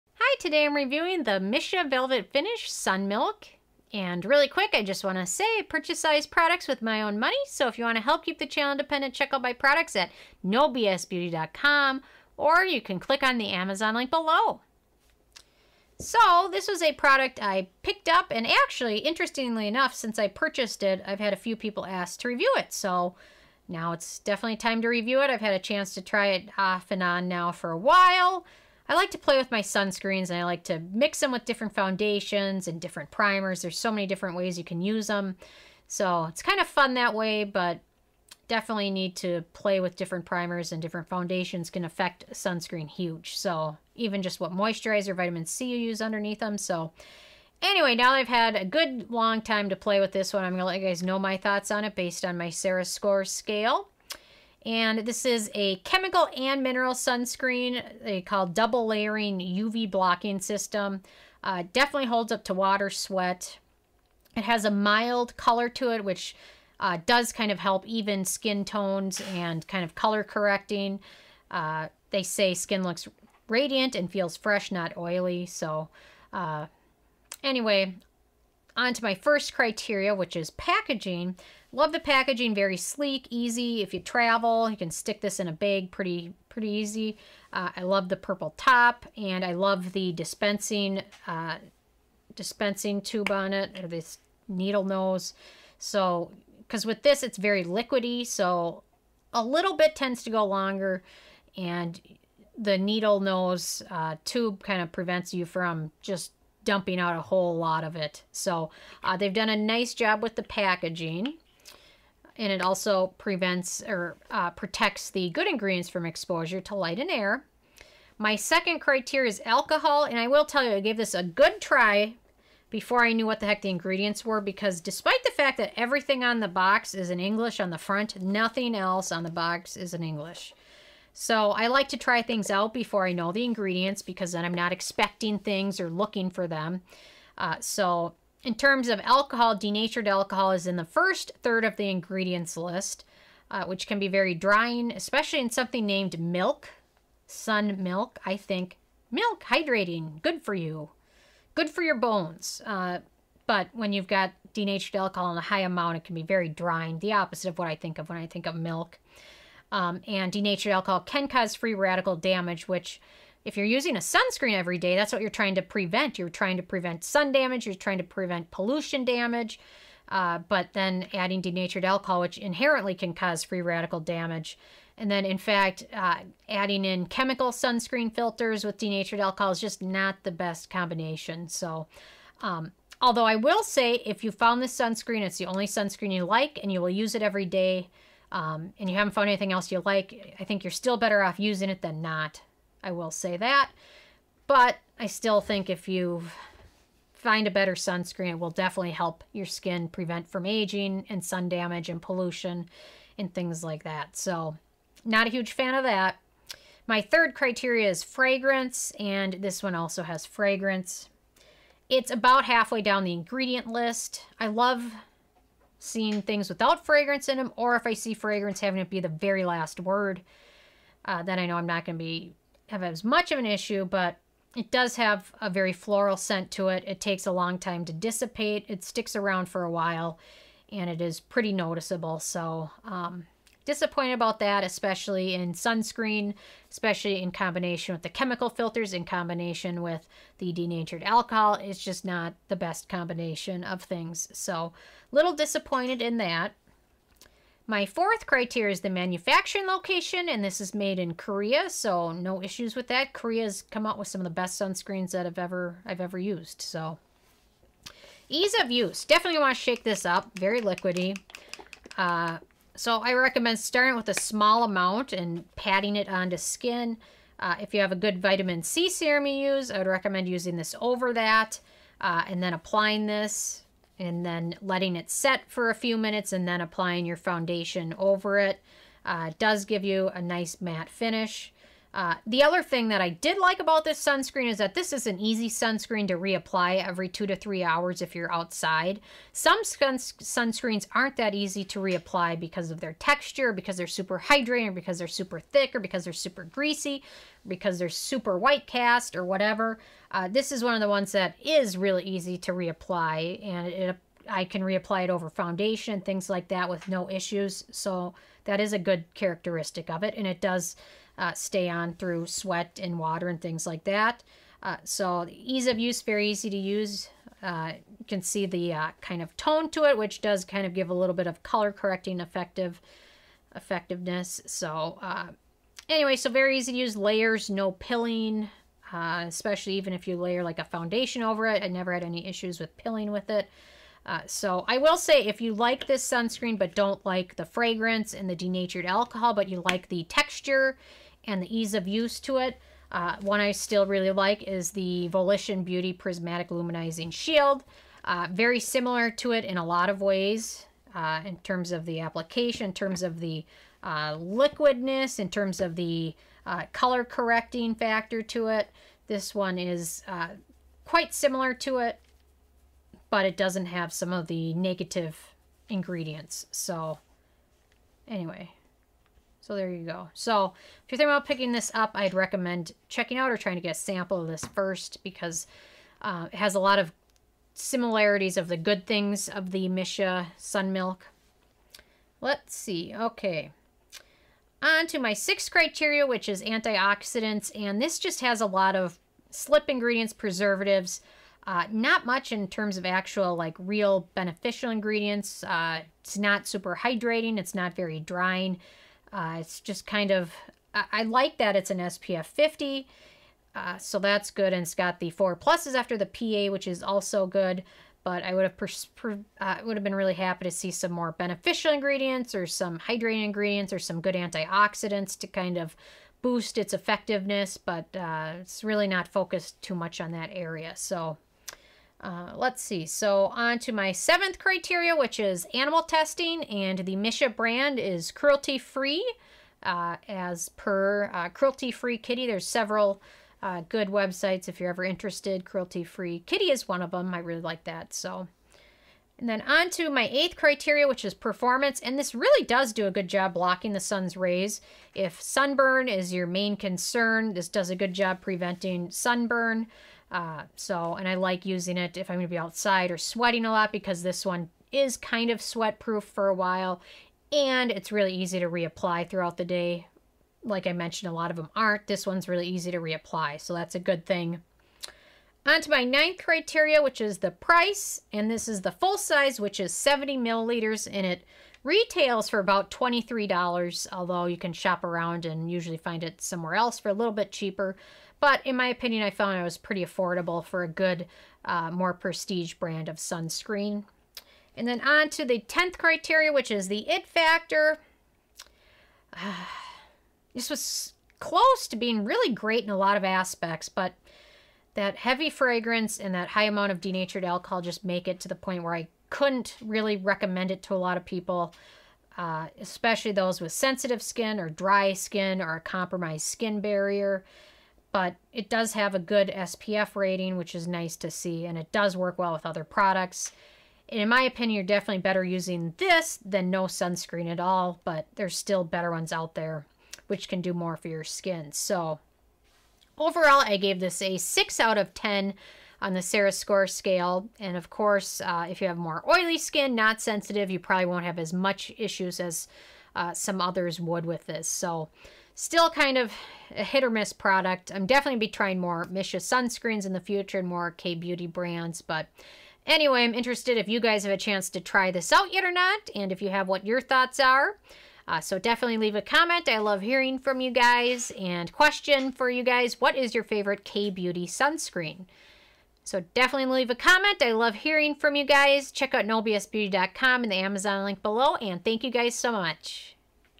Missha ☀️🌞⛅ All Around Safe Block Soft Finish Sun Milk SPF50+/PA+++
The Missha All Around Safe Block Soft Finish Sun Milk is a very easy to use sunblock, and it fits into my morning routine very easily.  It also layers well and has a super light liquid texture.  It has a light tint to it which helps to cancel out any redness to your skin tone.   I love that this is an SPF 50 and provides great protection.  What I didn't like is the high amount of fragrance and denatured alcohol.  Although, if you like this sunscreen you are still better off using a great sunscreen every single day, than trying to find one without fragrance or alcohol.

Missha All Around Safe Block Soft Finish Sun Milk EX SPF50+/PA+++
My Products:
No BS Beauty Travel Set
No BS Beauty Airless Jars
No BS Beauty Color Switcher
No BS Beauty Makeup Mixing Pallet